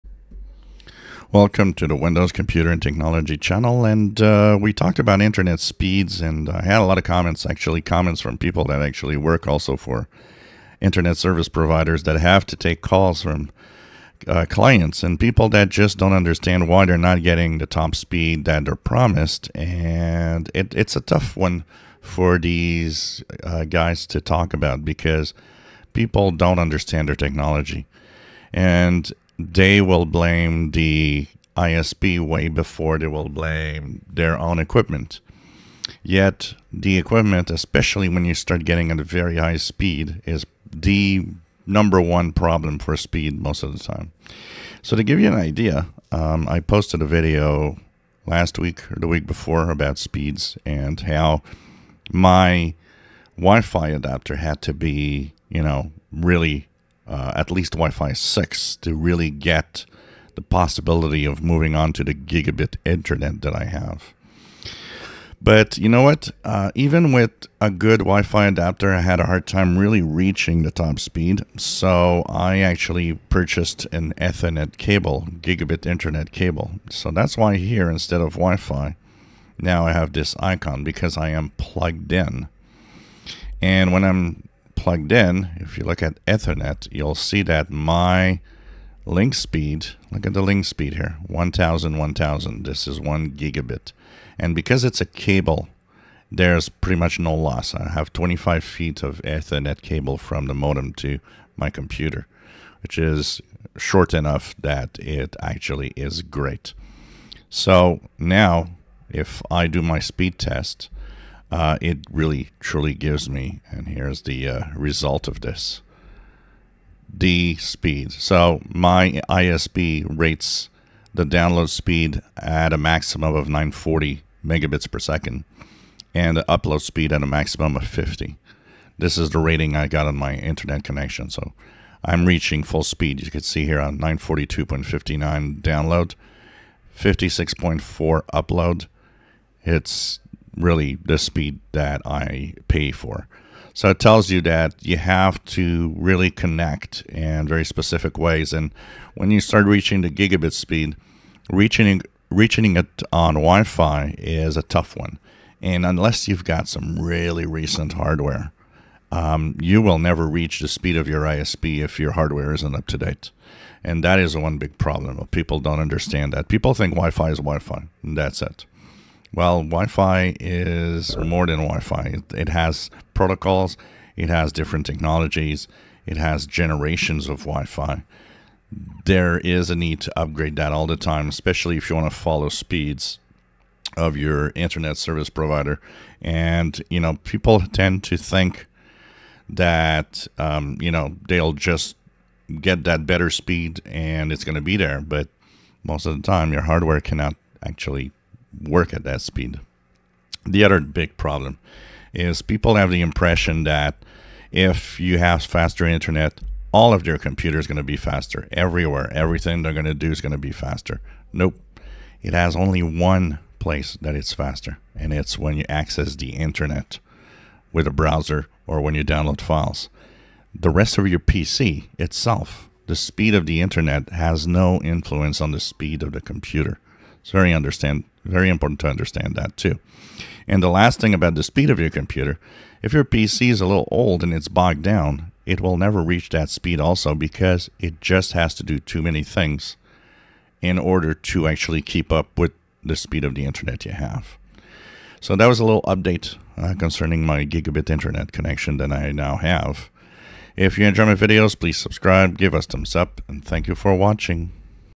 Internet speed and your Hardware part 2 Ethernet is king
Up to date hardware and sometimes Ethernet connection are what you need to reach maximum ISP speeds on Gigabit connections